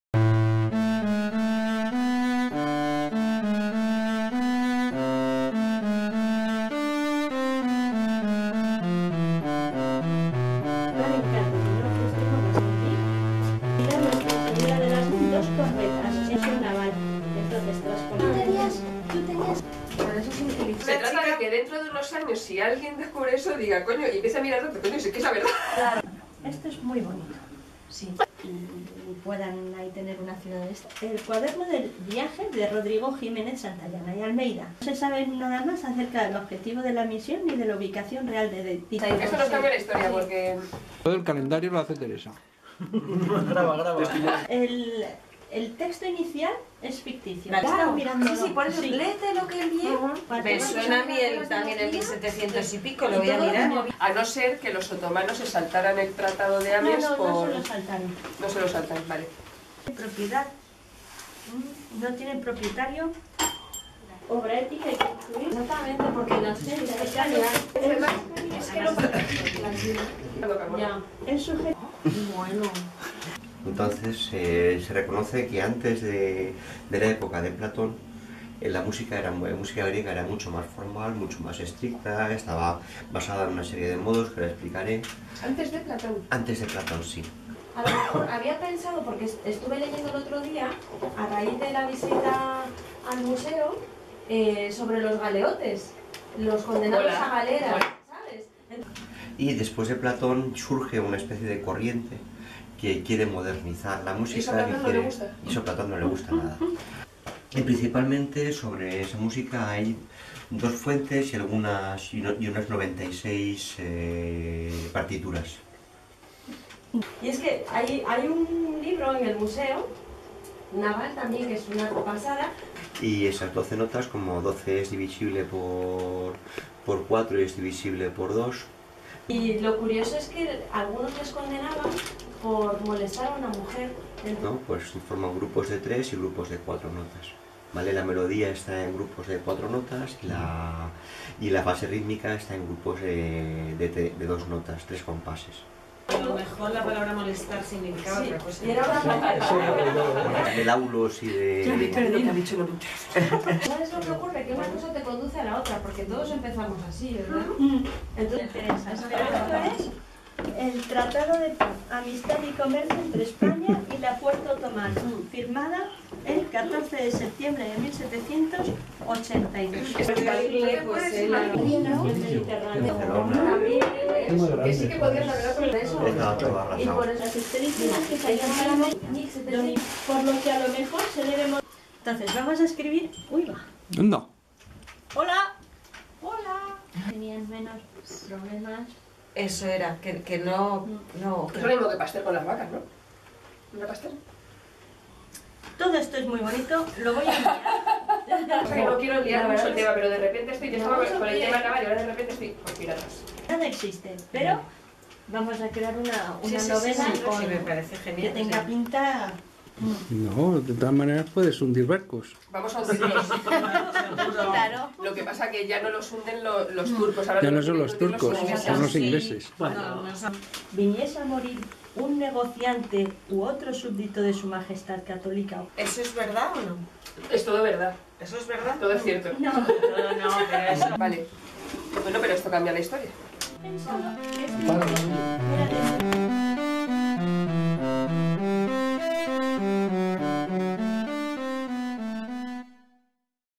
Encuentros en Bheiran en Diciembre en EL TROTAJUEVES Librería Asociativa
De viajes y Relatos, dedicada a ofrecerte la literatura y el viaje en un mismo regalo.
SEGUIMOS INVESTIGANDO, INTRIGANDO, EMOCIONÁNDONOS,
Vente a conocer la ciudad estado de Behirán
Encuentros en Behirán con el maravilloso país que fue BHEIRAN mientras descubrimos con asombro la enorme labor ilustradora de los investigadores Españoles en el siglo XVIII.
Seguimos RECONOCIÉNDOLOS, seguimos ADMIRÁNDOLOS.
Y es por eso que volvemos a reunirnos en las “Cuevas del Dios Japones en Carabanchel” para continuar con una ilusión, con un sueño que ya empieza a ser de más.